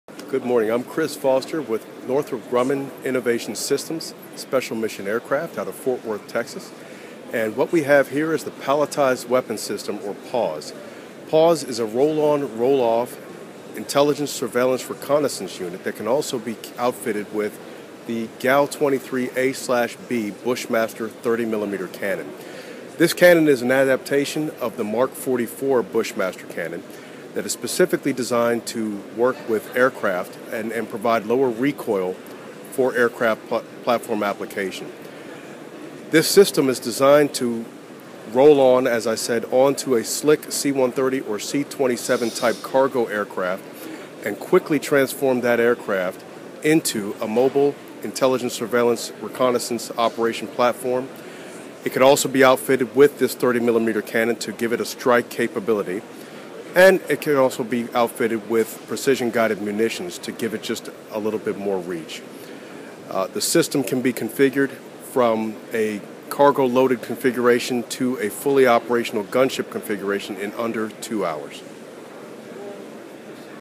Byron Foster, Sr Business Development Rep Northrop Grumman briefing on PAWS to Chaitali Bag ADU
Byron Foster, Sr. Business Development Rep. Northrop Grumman briefing on PAWS to Chaitali Bag ADU at IDEX 2019